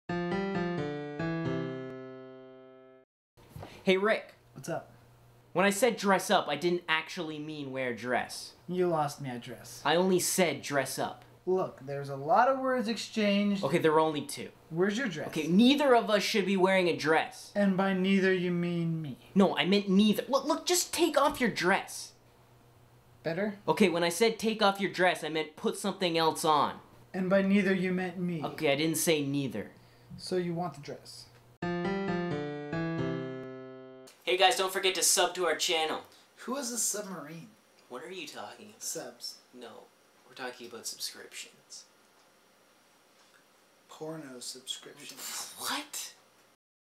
Epi 15: Dress Up
Matt tells Rick to dress up but he gets confused.

***New Episodes of Matt and Rick Every Monday***